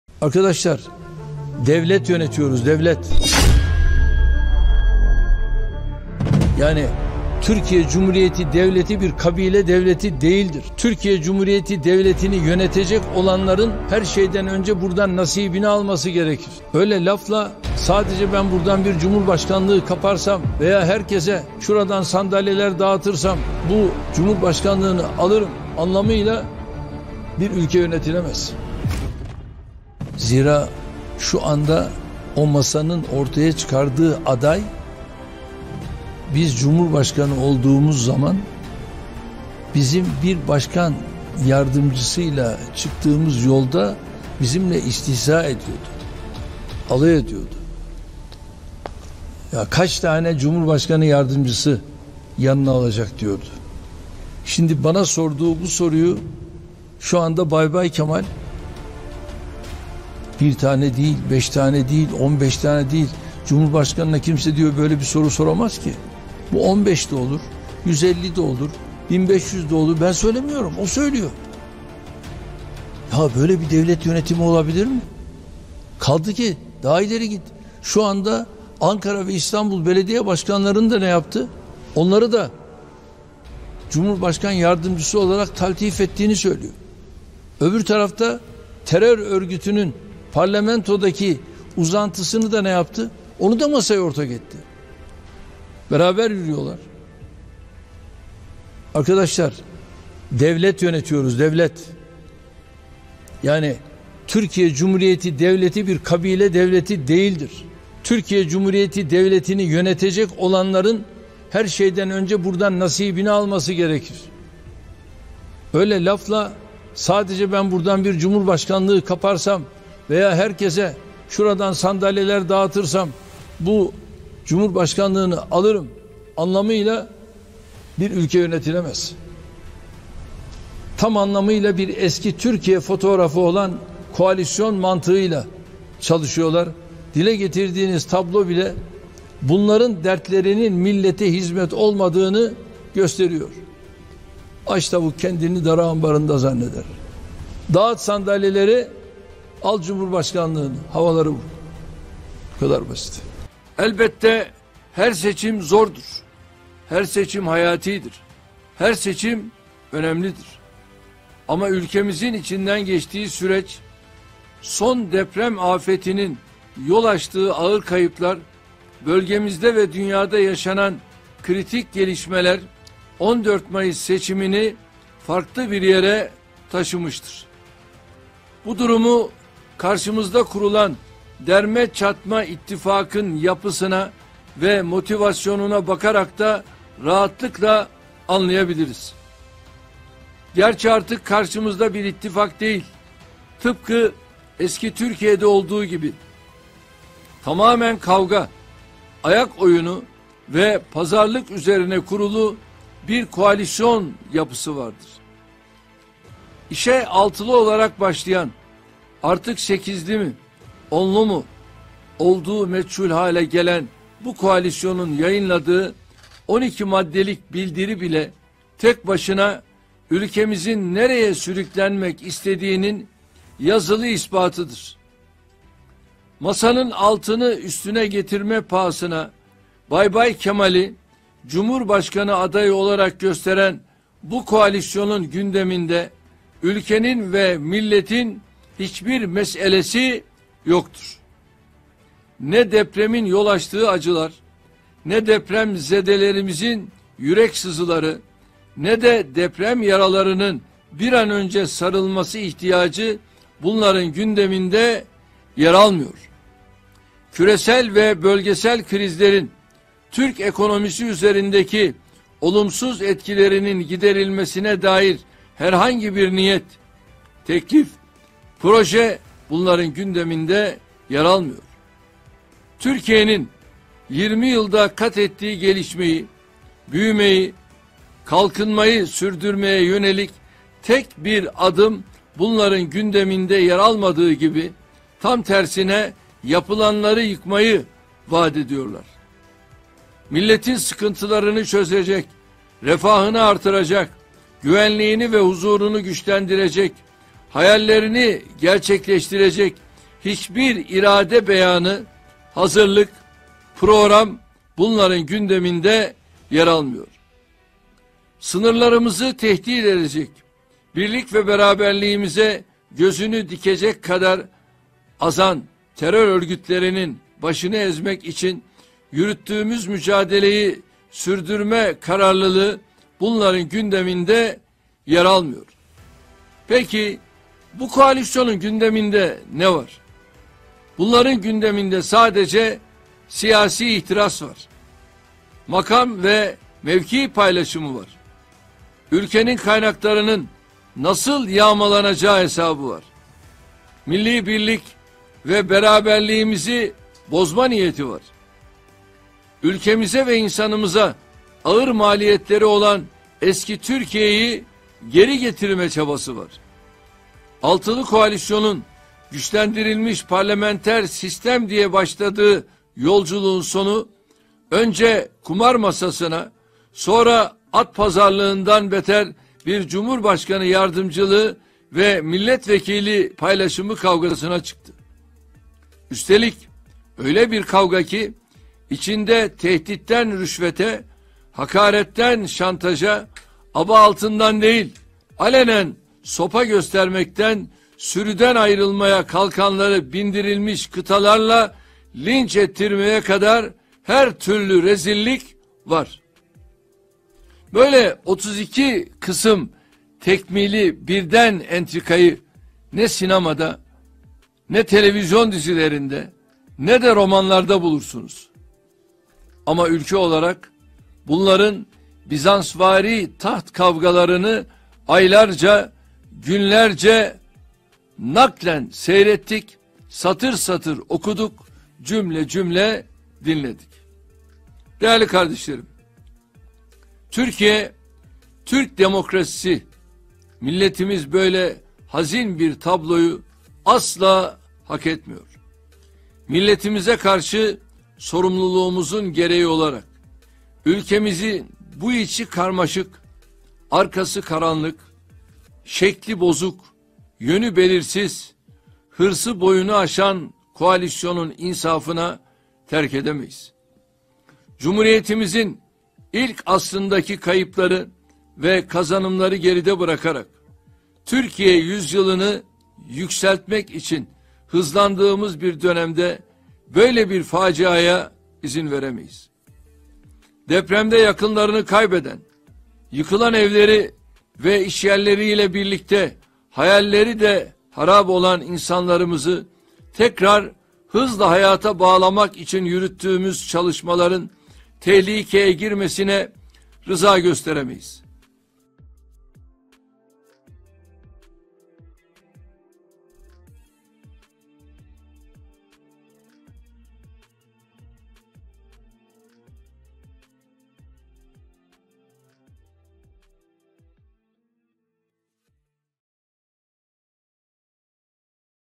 DEVLET YÖNETİYORUZ DEVLET! Erdoğan Kemal'in Ciddiyetsizliğe Dayanamadı! Fena Çaktı!
Erdoğan Kılıçdaroğlu'nun yardımcı sürüsüne gereken cevabı böyle verdi.